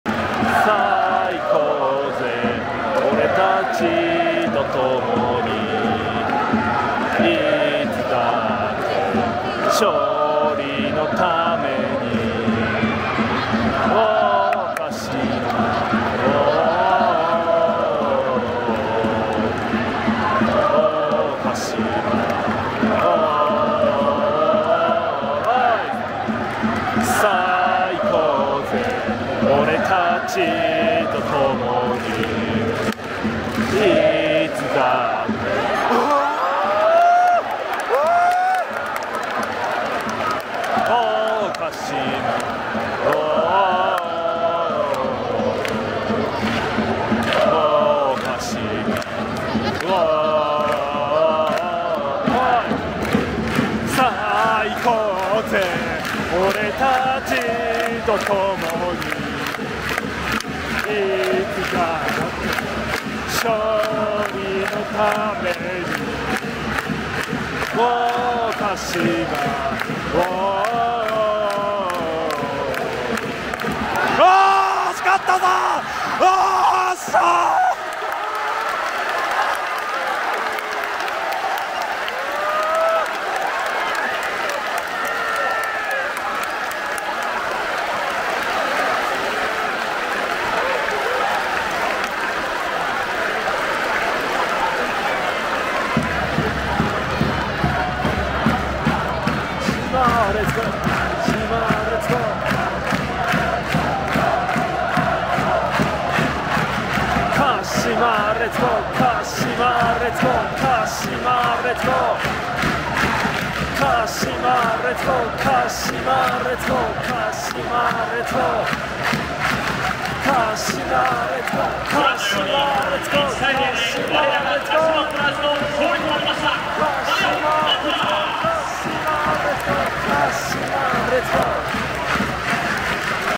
[観戦動画 先出し映像] 試合終盤・勝利の為に～勝利の瞬間！ 鹿島アントラーズ vs ヴィッセル神戸 2024 Jリーグ J1 J2 J3
やったぜ！